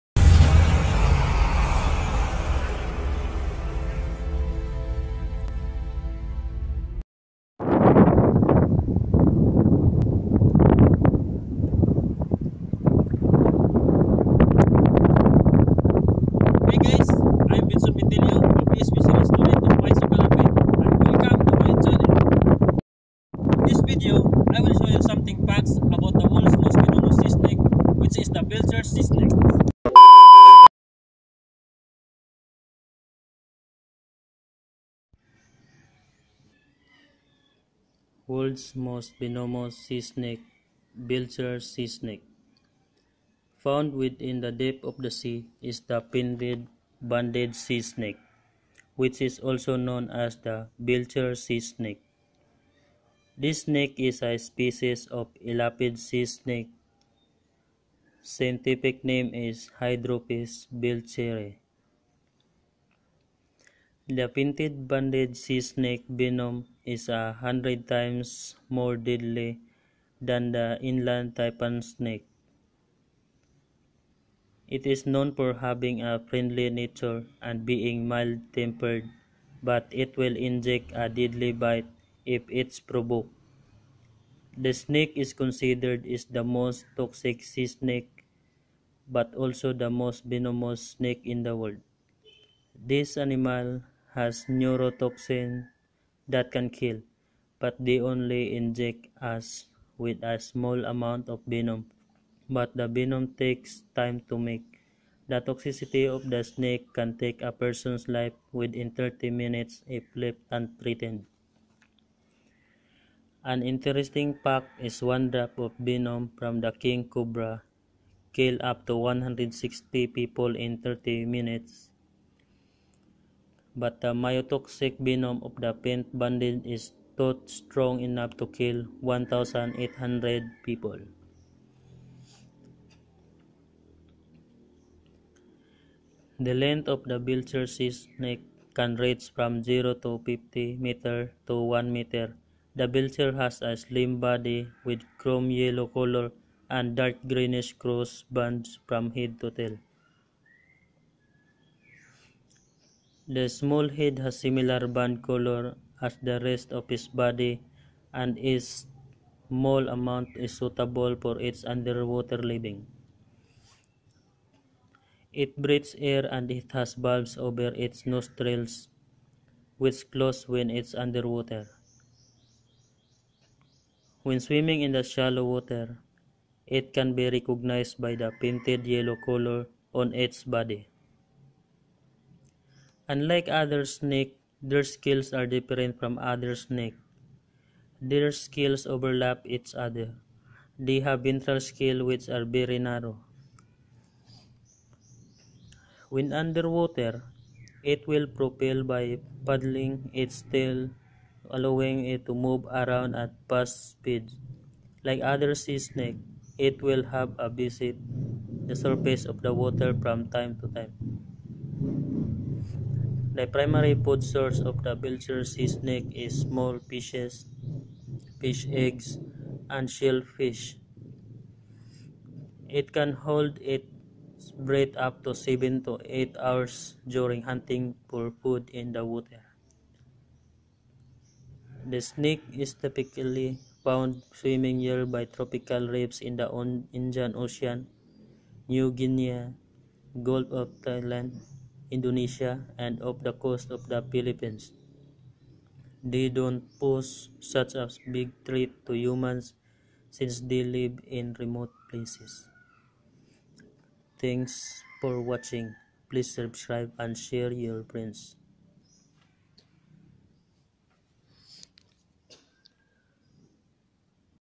Facts About the Worlds Most Venomous Sea Snake The Belcher Sea snake
The belcher's sea snake is of moderate size, ranging from 0.5 to 1 meter (about 20–40 inches) in adult length. Its thin body is usually chrome yellowish in colour with dark greenish crossbands. The dorsal pattern does not extend onto the venter. The head is short and has bands of the same colours. Its mouth is very small but suitable for aquatic life. Its body, when viewed out of water, appears to have a faint yellow colour. Its scales are different from most other snakes in that they overlap each other. Each dorsal scale has a central tubercle. The body is strongly laterally compressed posteriorly. The ventral scales are very narrow, only slightly wider than the dorsal scales.

Animal Planet
Belcher Sea Snake#Worlds most Venomous sea snake
Marine Biology
Venomous Snake
National Geographic
Animal Science